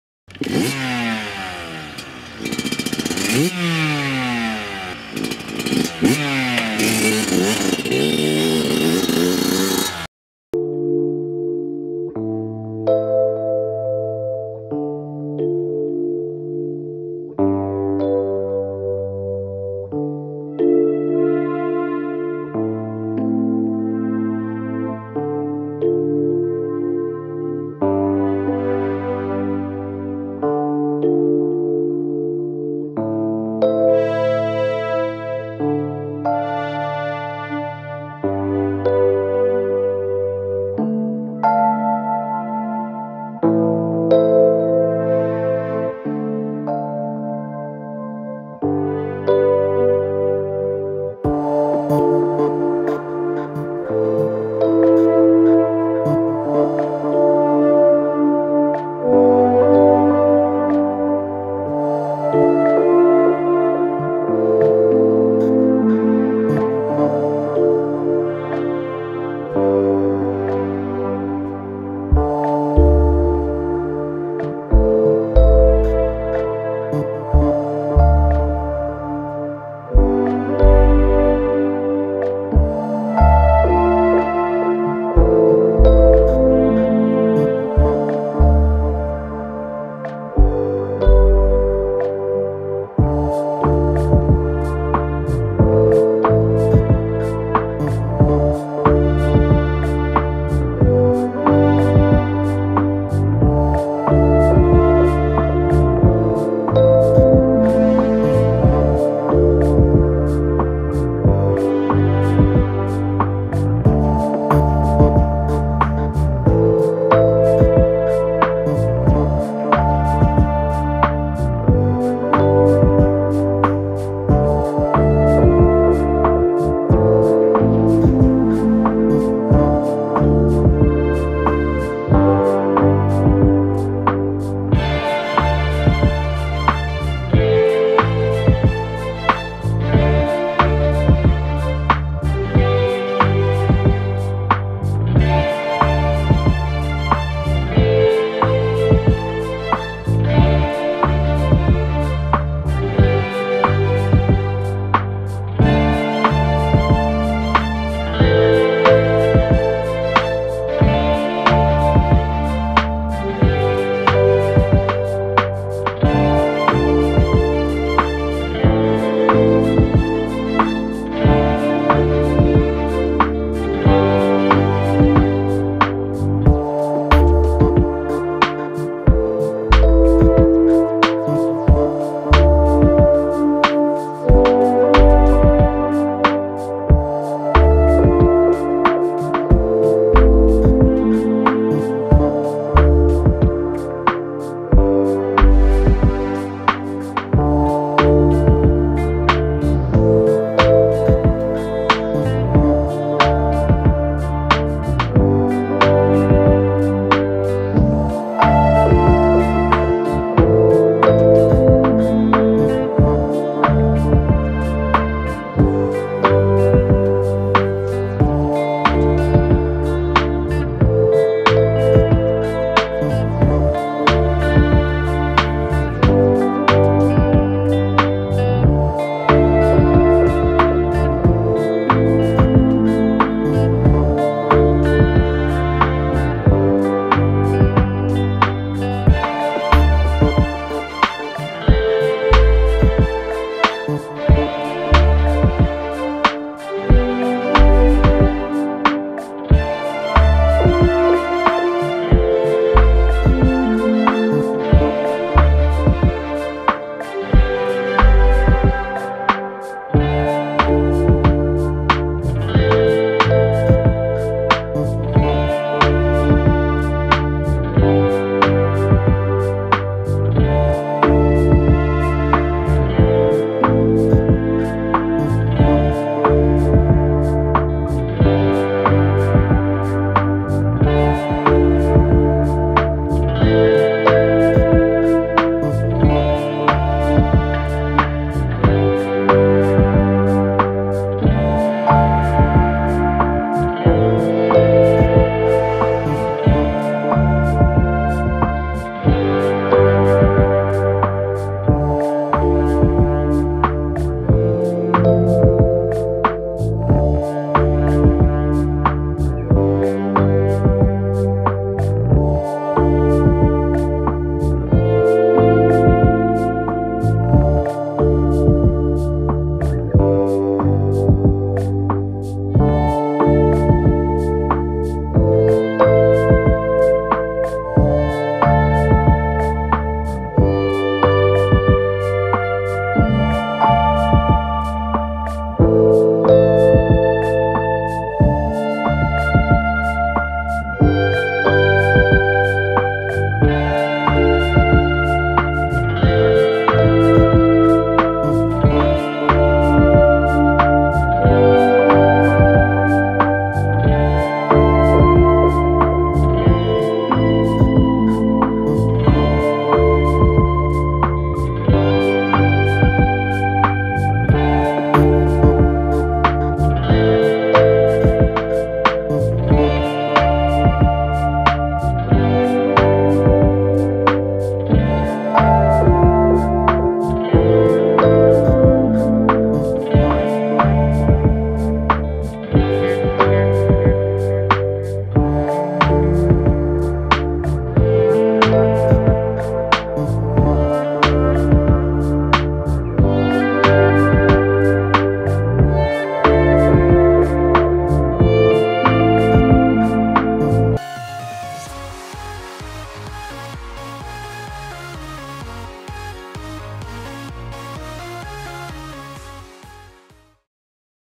JUAL CEPAT HONDA VARIO 125 2016 MURAH
Dijual cepat 1 unit motor honda vario 125 CBS 2016,
motor tgn1 an: istri ane dari baru letter AD boyolali,
kondisi standar,  sedikit baret2 halus pemakaian.
jual murah saja 13,500,000 pas yak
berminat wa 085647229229